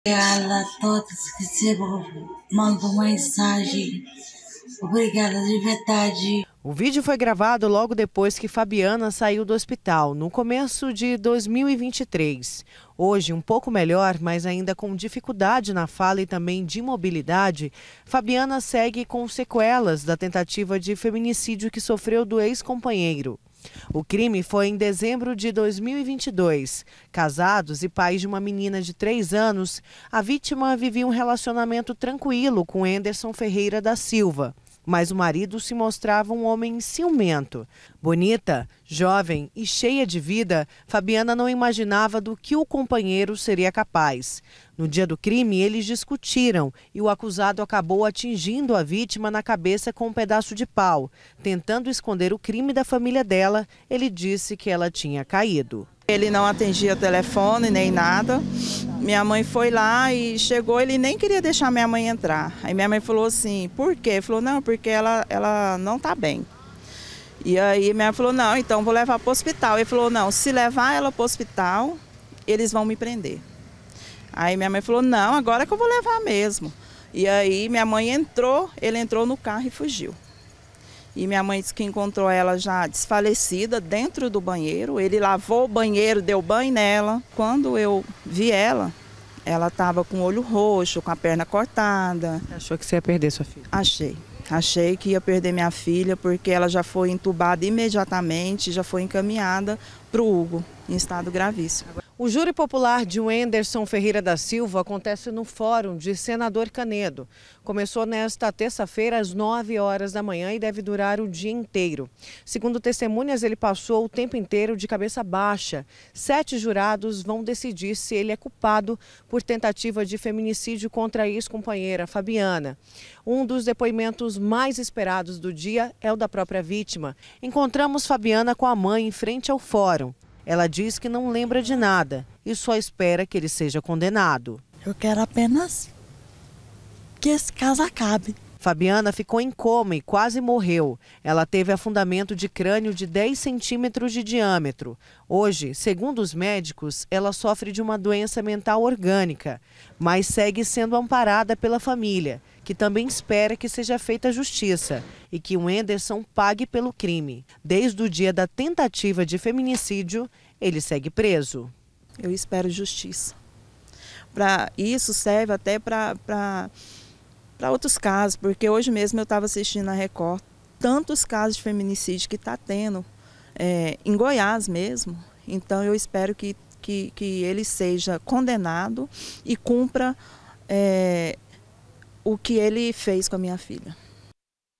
FAMÍLIA ESPERA CONDENAÇÃO: HOMEM VAI A JÚRI POPULAR POR ESPANCAR A EX
UM HOMEM QUE ESPANCOU E DEIXOU A EX-COMPANHEIRA EM COMA, EM 2022, ESTÁ SENDO JULGADO HOJE, NO FÓRUM DE SENADOR CANEDO/ FELIZMENTE A MULHER SOBREVIVEU, MAS ATÉ HOJE ENFRENTA AS SEQUELAS  DA AGRESSÃO.